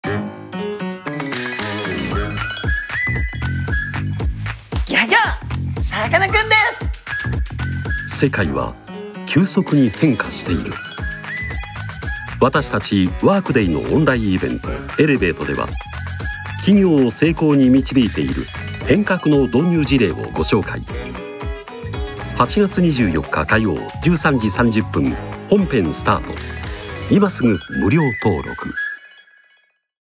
【 今すぐ無料登録 】ワークデイのオンラインイベント - ８月２４日 (火) １３時３０分 本編スタート
皆さまのご参加をお待ちしております！

企業の人事、IT(情報システムやデジタル)、財務や会計部門のリーダーが現況に立ち向かい、変化する時代への準備を整える上で必要なことと、企業や専門家の事例を中心にWorkday のエンタープライズ マネジメント クラウドがどのように役立つかについてご紹介します。